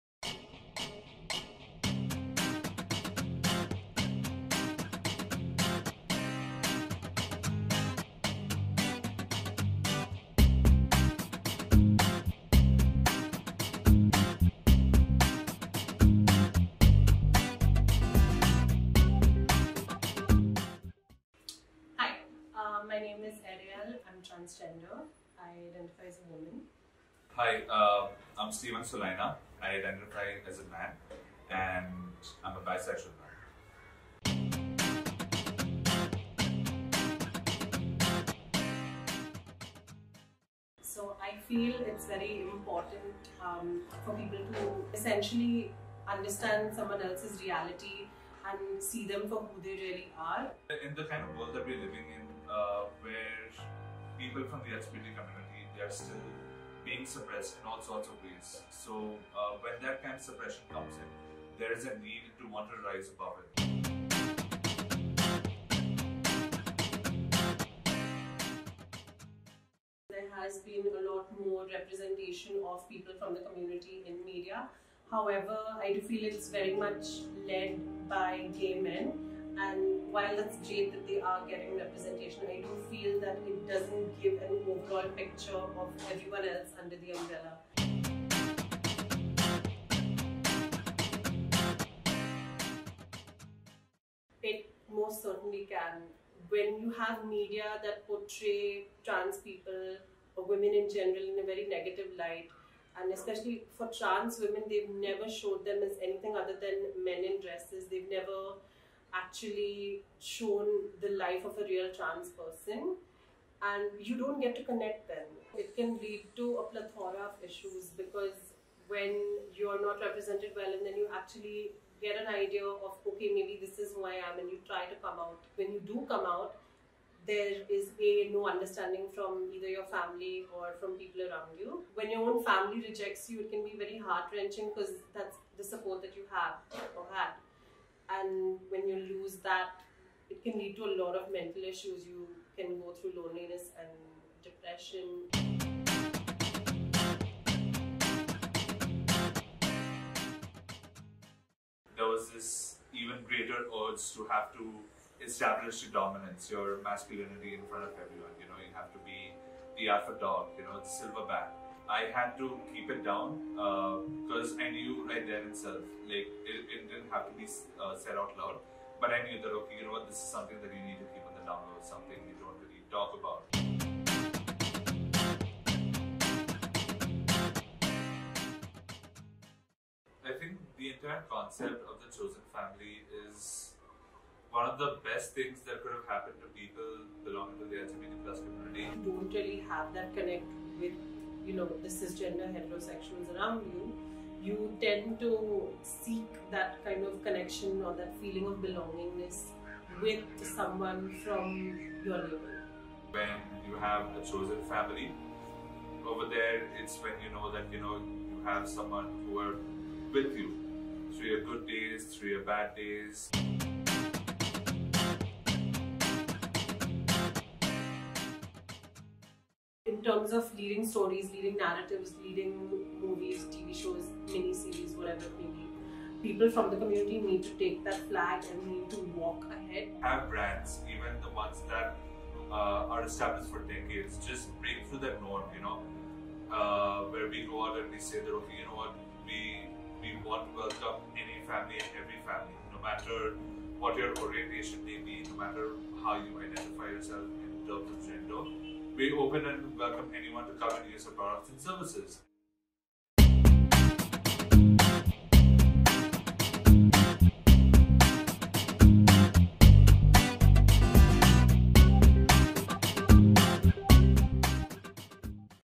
The Butt of All Jokes
Today, in keeping with the spirit of Transgender Day of Remembrance, we talk about not the ones we lost but the transgender lives that are still being suppressed and maligned by their misrepresentation in the digital media of our country. Watch this video to understand more about how people from the LGBTQIA++ community still suffer from stigmatisation and alienation in the Indian society.